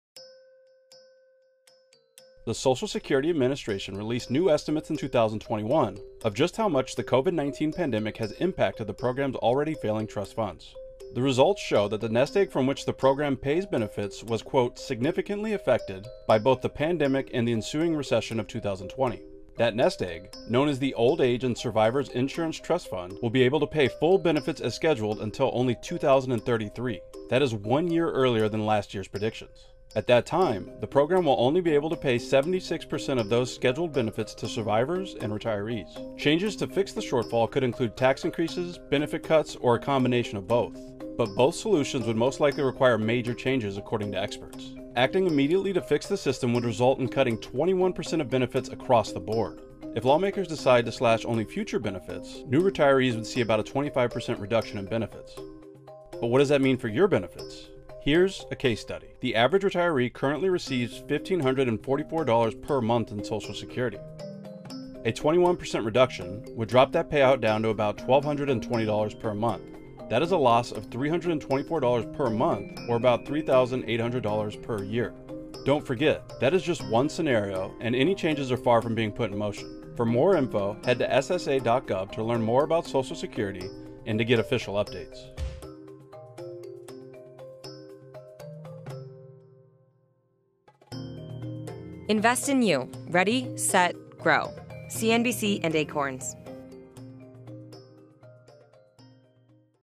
Here's what could happen to your benefits if Social Security runs out of money
The Social Security Administration released new estimates in September 2021 of just how much the Covid-19 pandemic has impacted the program's already ailing trust funds. Major solutions to fix the Social Security shortfall will most likely need to come from increasing taxes or slashing benefits. Here's a breakdown of one scenario.

The Social Security Administration needs more money.

The government agency’s latest report shows that the combined trust funds used to pay Social Security retirement, survivor and disability benefits will only be able to pay out as scheduled through 2033.

After that, the program will be able to pay 78% of benefits as scheduled.

Major solutions to fix the shortfall will most likely need to come from increasing taxes or slashing benefits.

Check out this video for a case study based on a solution that will leave retirees with less money.